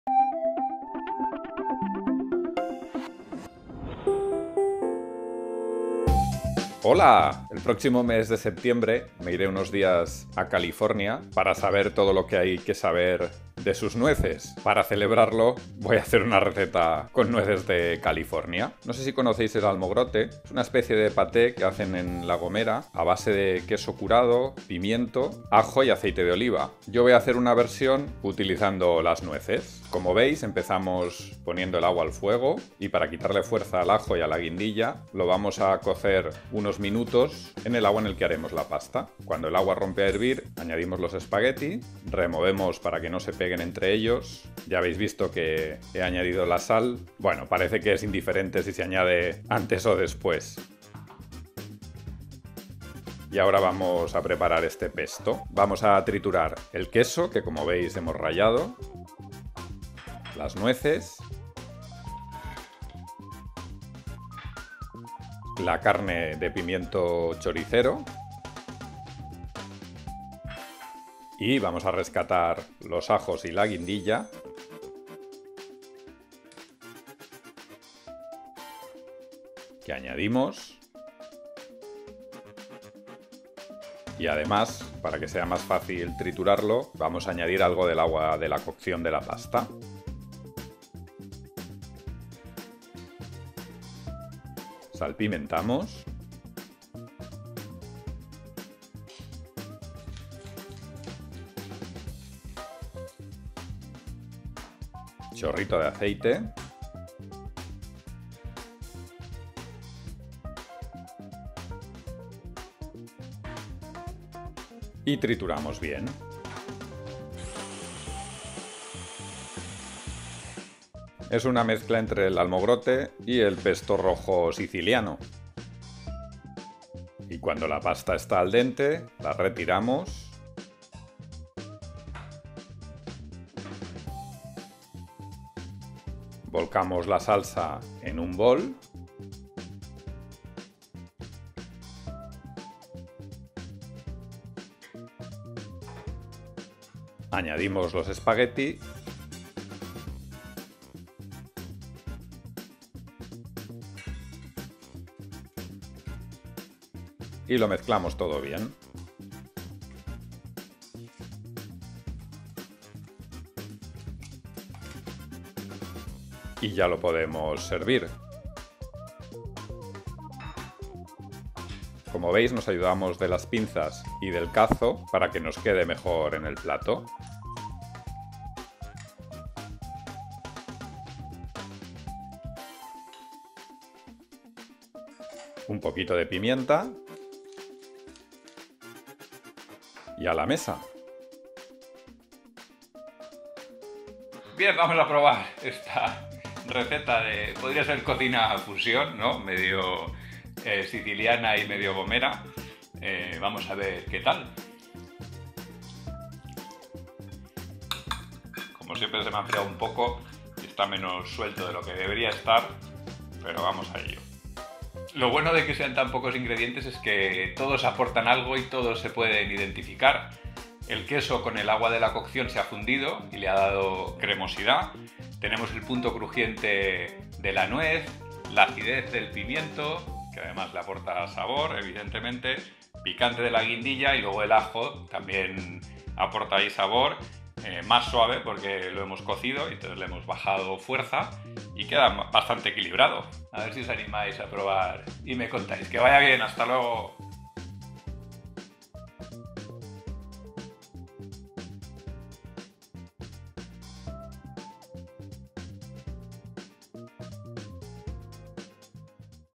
Spaghetti con pesto gomero
Unos espaguetis o spaghetti con una mezcla entre el pesto siciliano y el almogrote gomero. Un mix con nueces que ha quedado bien.

Ingredientes:
500 g de spaghetti
2 l de agua
8 g de sal
200 g de queso curado
150 g de nueces
30 g de carne de pimiento choricero
4 dientes de ajo
1 guindilla seca
Aceite de oliva virgen extra
Pimienta negra
Sal